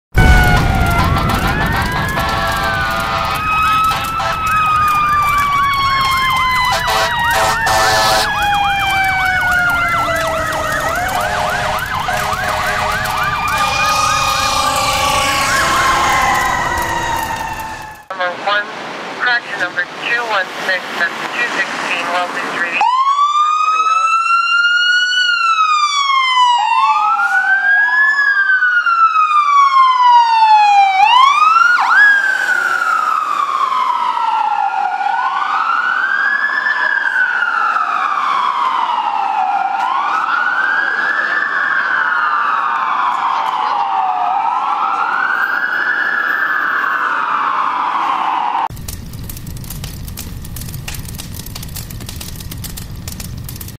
Toronto Fire - C33 & P332 Responding
Toronto Fire Services, District Chief 33 and Pumper 332 responding.